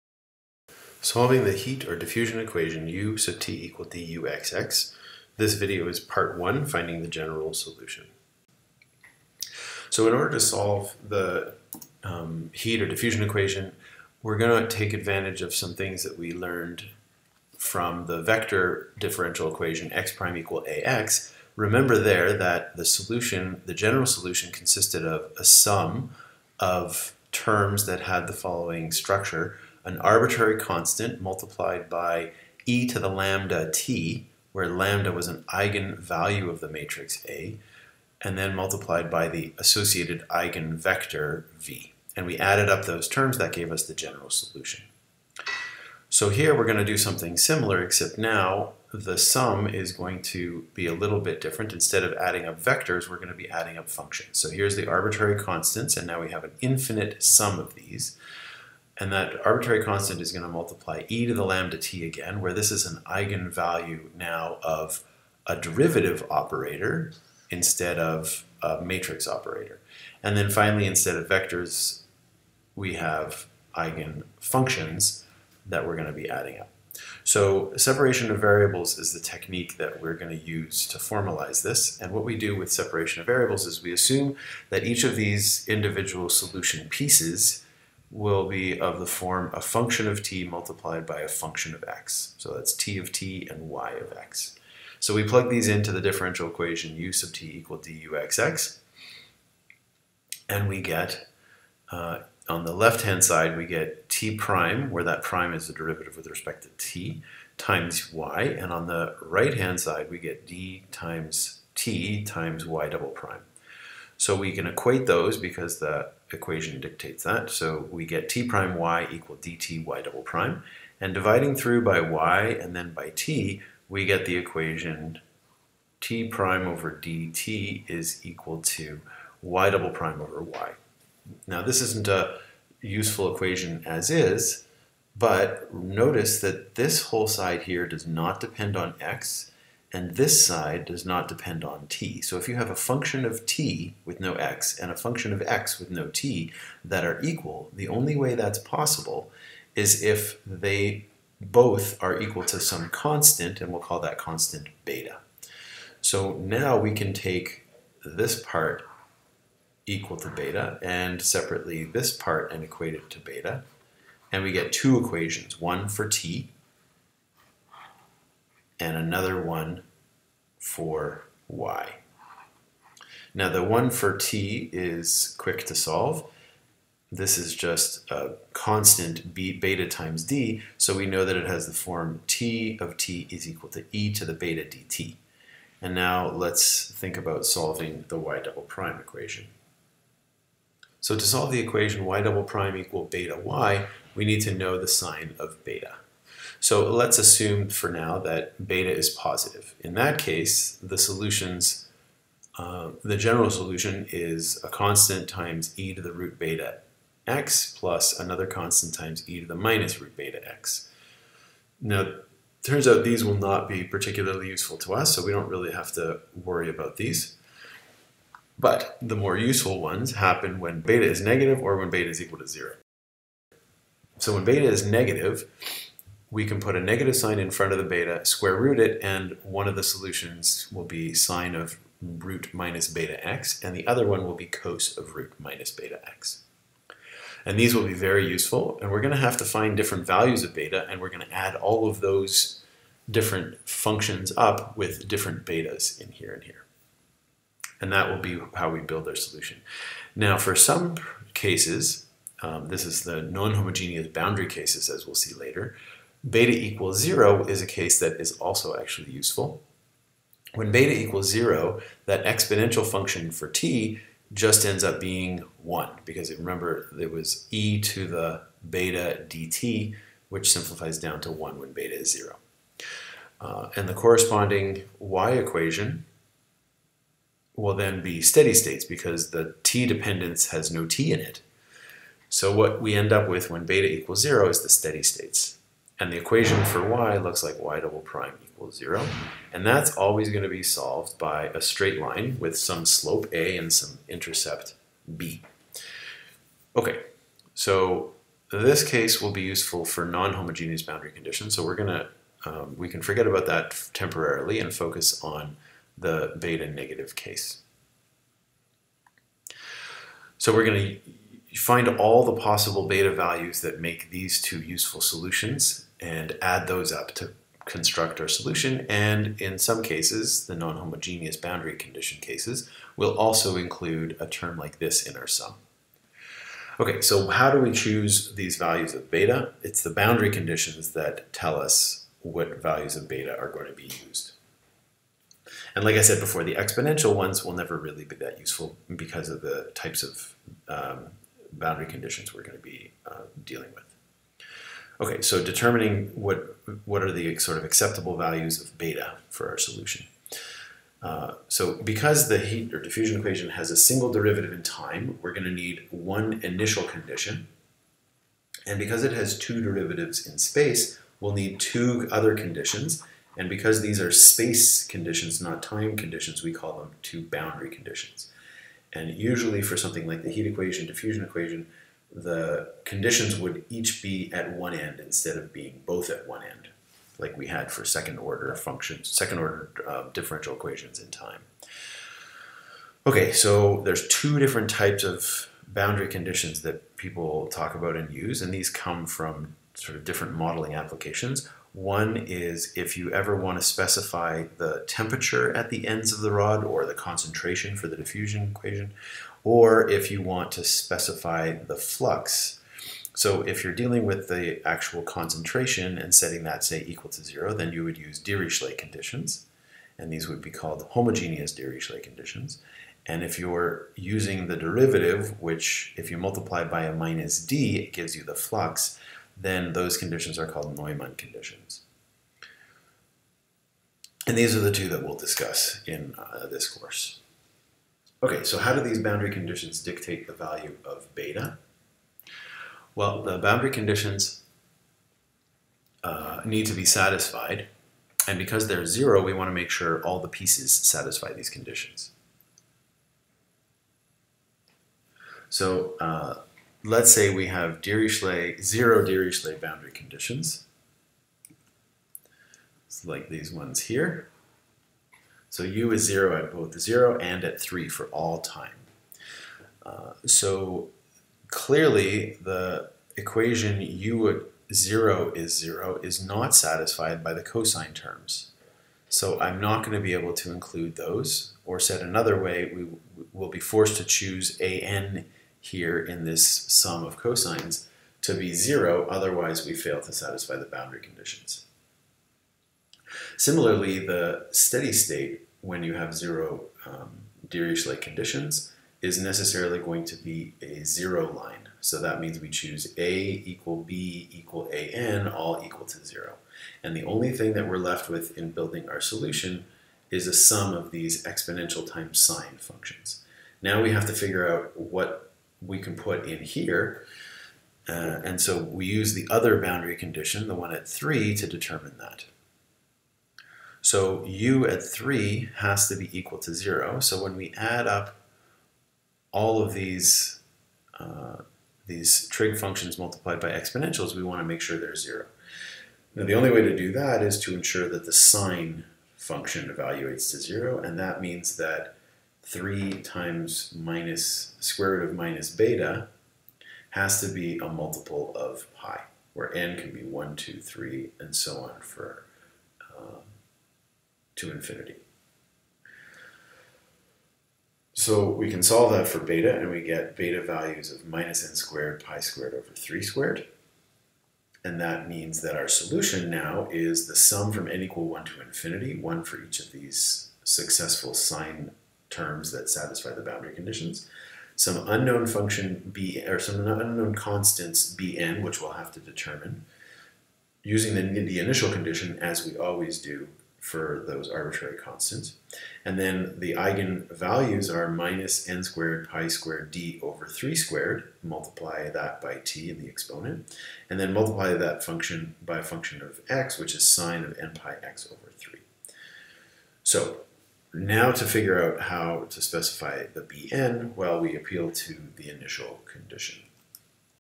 Solving the Heat/Diffusion equation - part 1
In this video, I explain how to find the general solution to the Heat/Diffusion equation.